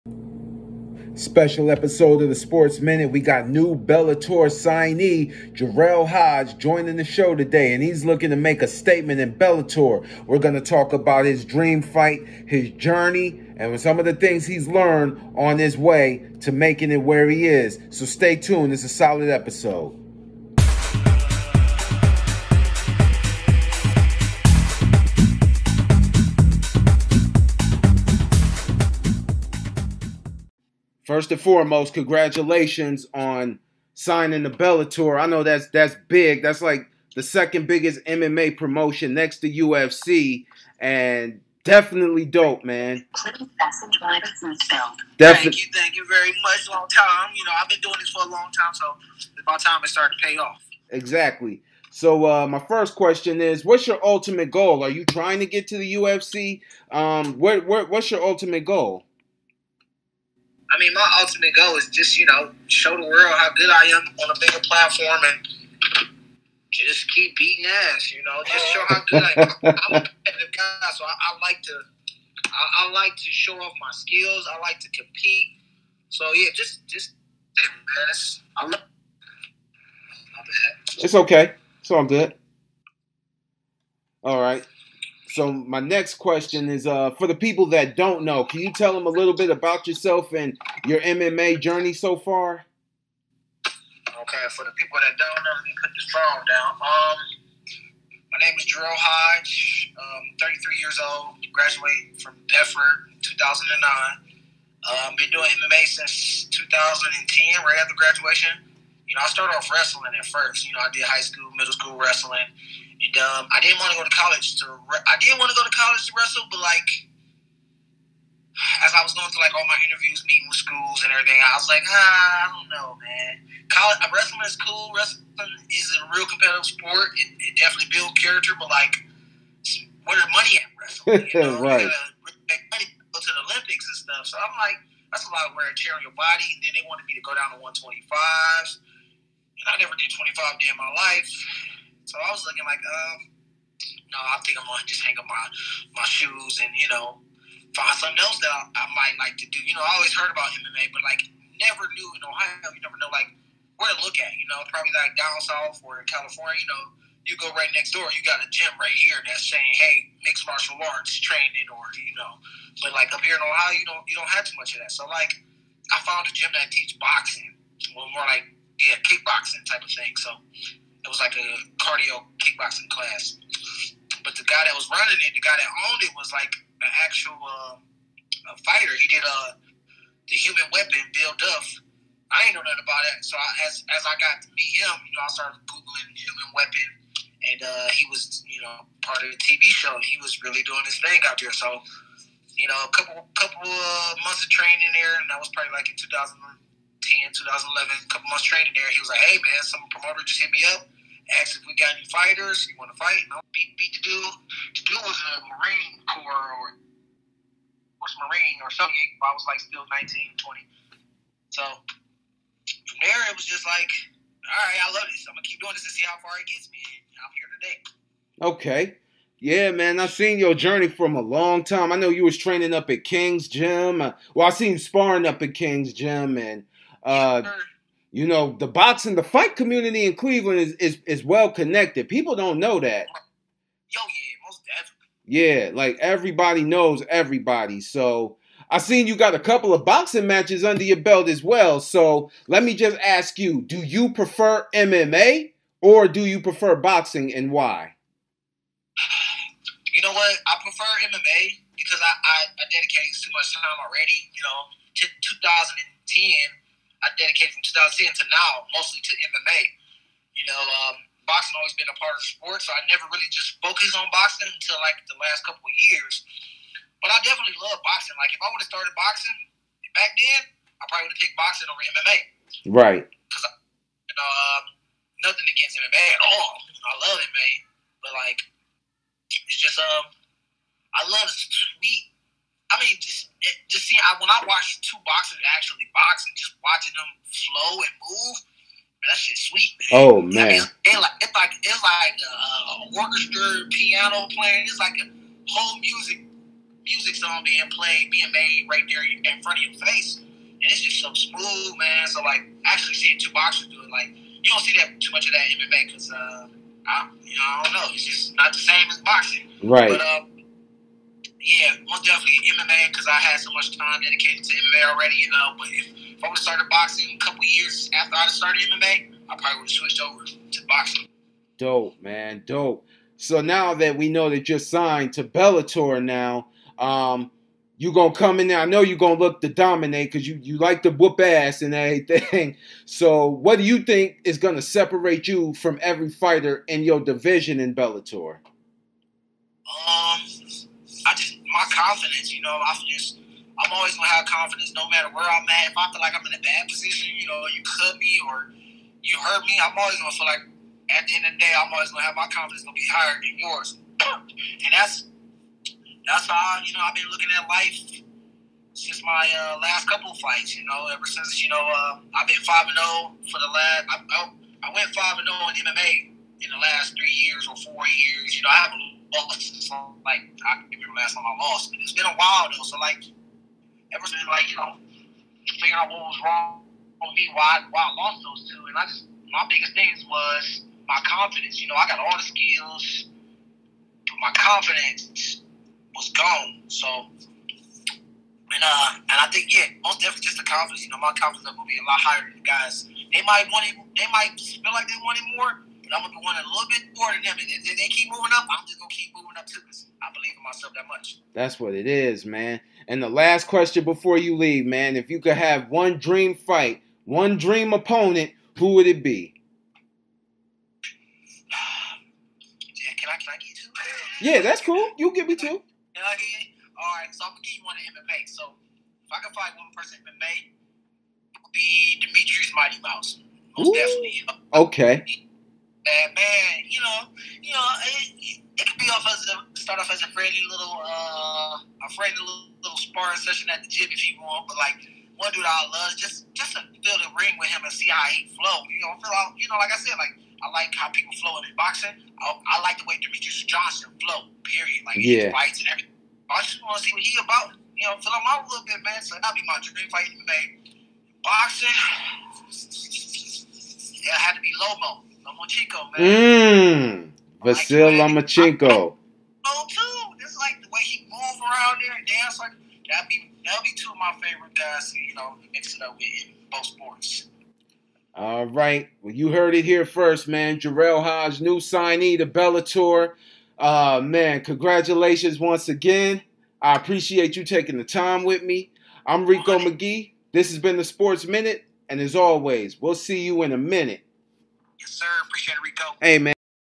MMA fighters better watch out
New Bellator Signee Jerrell Hodge Joins the show to talk about his goals, his lessons and his Journey in MMA